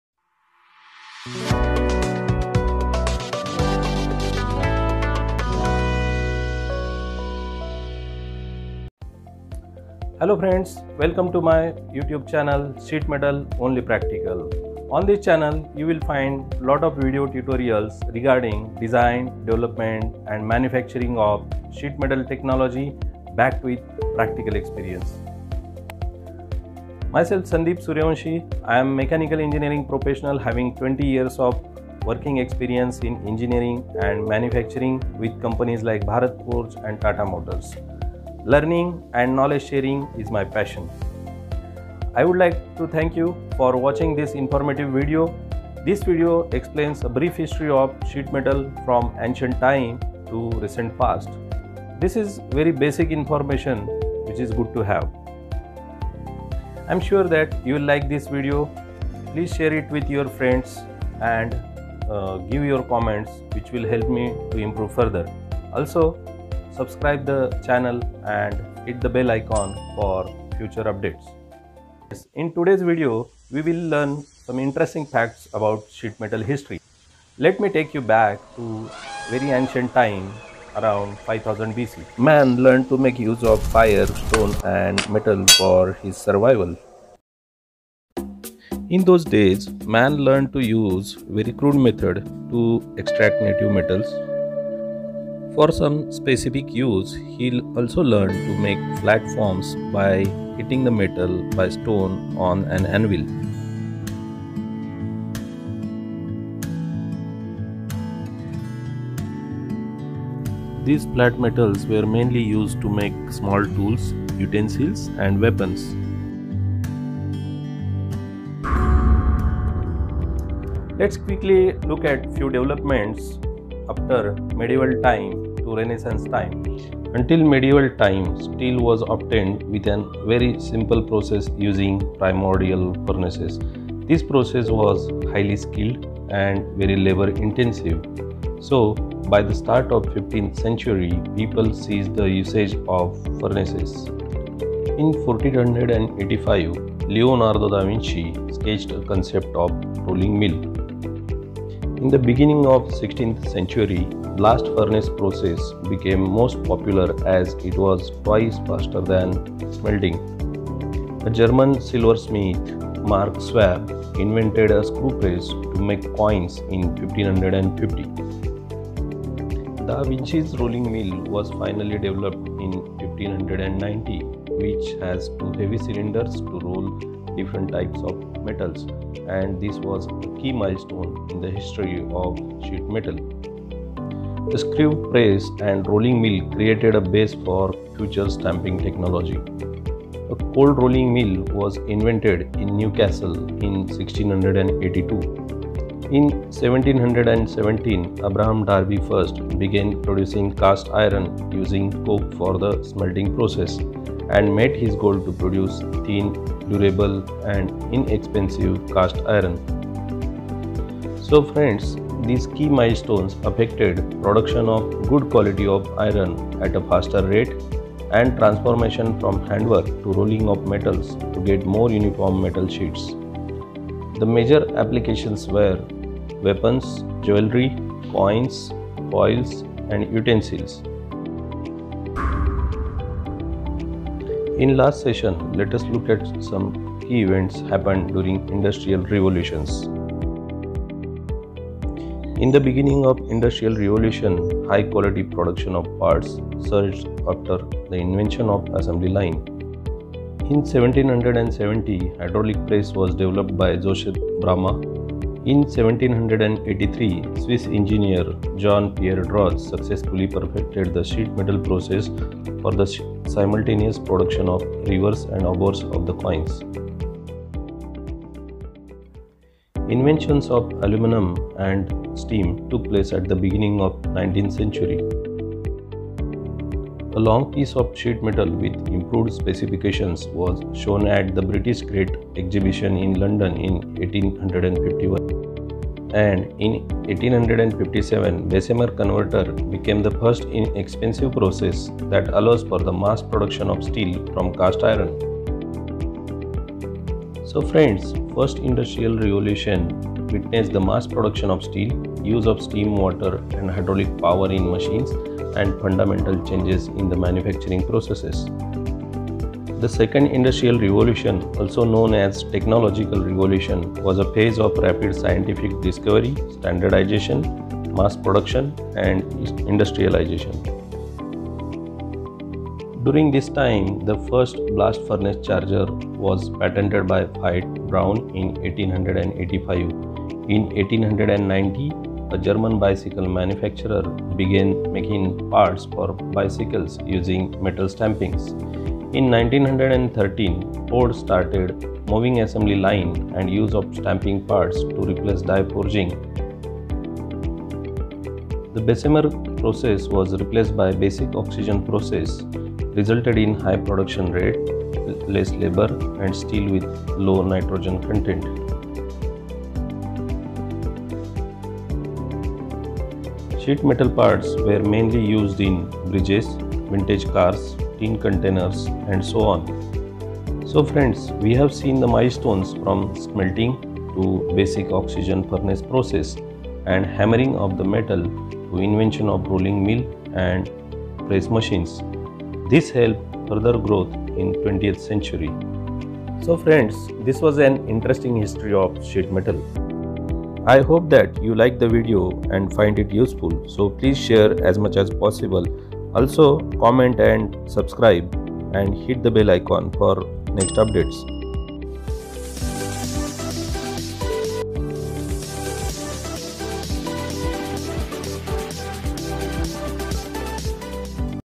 History of Sheet Metal - Basic 1 | A brief introduction of sheet metal
This is a very basic information about history of sheet metal technology development from ancient time around 5000 BC to 20th century. It lists all milestones related to metallurgical highlights of steel production and inventions of rolling mill, press machines with other events.

You will learn that how sheet metal technology development from ancient time to medieval period and during industrial revolutions.

It took me lot of time to research required for this video and created excitement to learn the historical events.